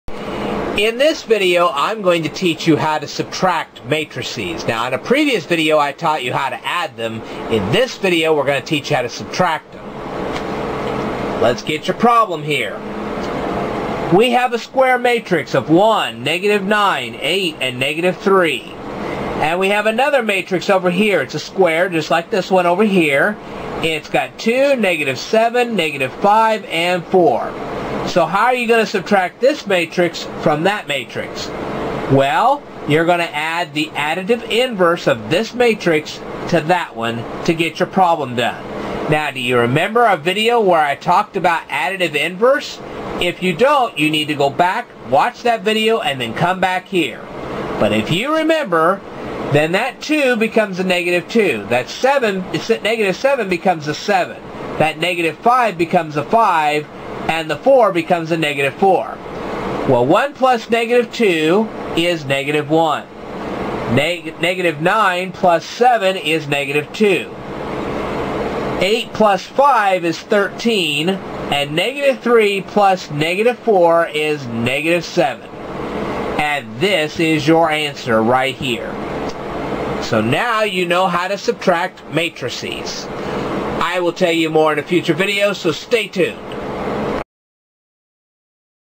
Subtracting Matricies
In this video I teach people how to subtract matricies.